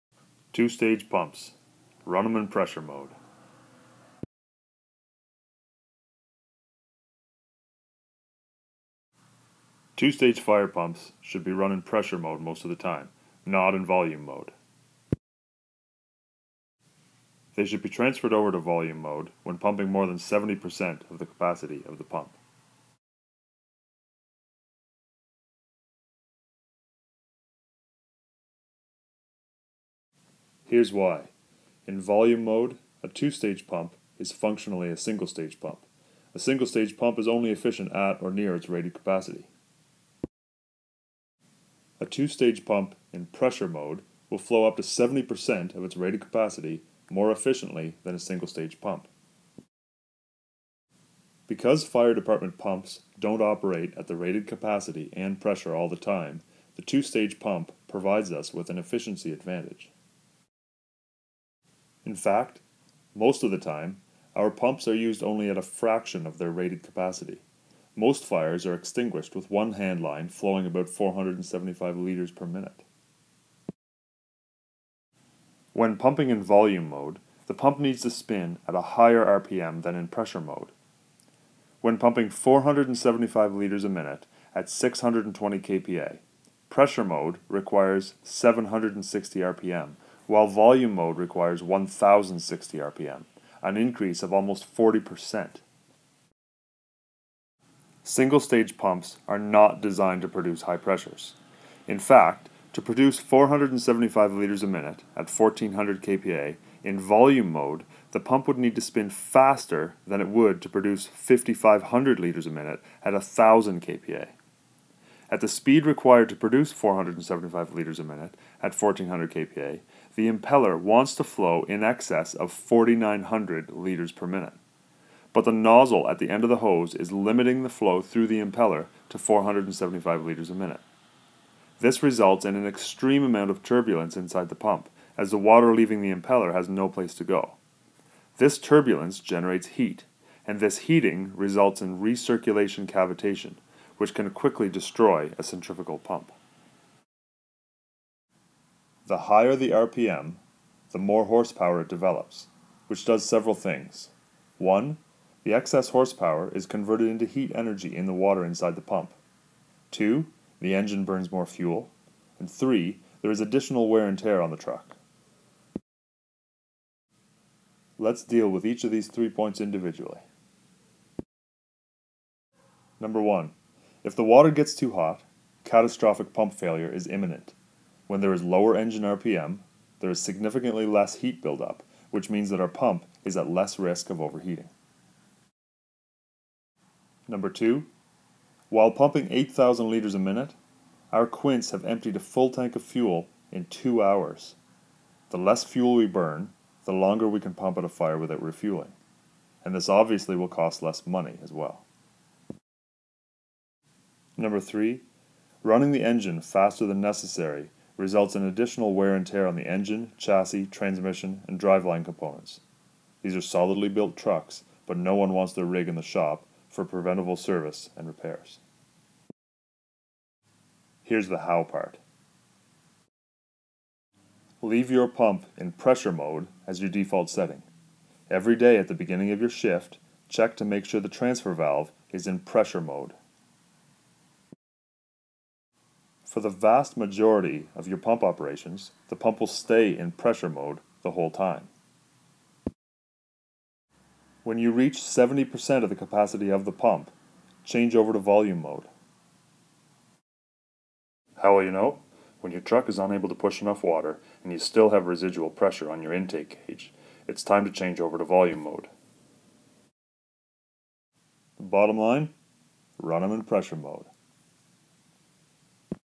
Two Stage Pumps (Pressure Mode)
An explanation of why two stage pumps on fire engines should be run in pressure mode most of the time.